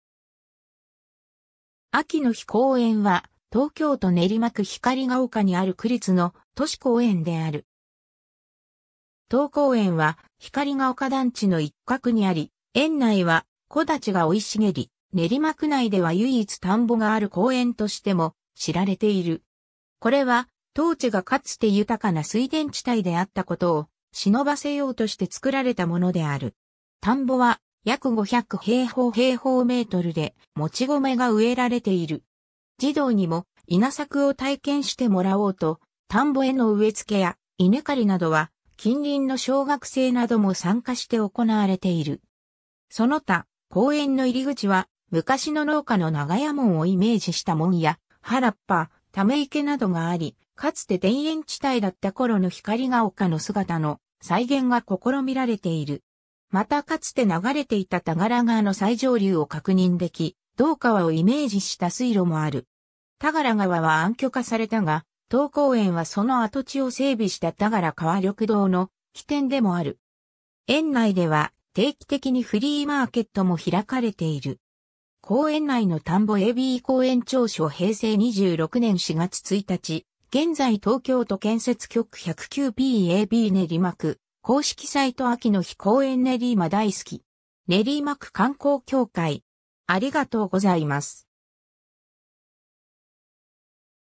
秋の陽公園
秋の陽公園（あきのひこうえん）は、東京都練馬区光が丘にある区立の都市公園である。
 当公園は光が丘団地の一角にあり、園内は木立が生い茂り、練馬区内では唯一田んぼがある公園としても知られている。
これは、当地がかつて豊かな水田地帯であったことをしのばせようとして造られたものである。
 田んぼは約500平方m2で、もち米が植えられている。
児童にも稲作を体験してもらおうと、田んぼへの植えつけや、稲刈りなどは近隣の小学生なども参加して行われている。
 その他、公園の入り口は昔の農家の長屋門をイメージした門や、原っぱ、ため池などがあり、かつて田園地帯だった頃の光が丘の姿の再現が試みられている。
またかつて流れていた田柄川の最上流を確認でき（源流は土支田の辺り）、同川をイメージした水路もある。
田柄川は暗渠化されたが、当公園はその跡地を整備した田柄川緑道（グリーンベルト）の起点でもある。
 園内では定期的にフリーマーケットも開かれている。
公園内の田んぼ ^ a b 公園調書　平成26年4月1日現在　東京都建設局　109p.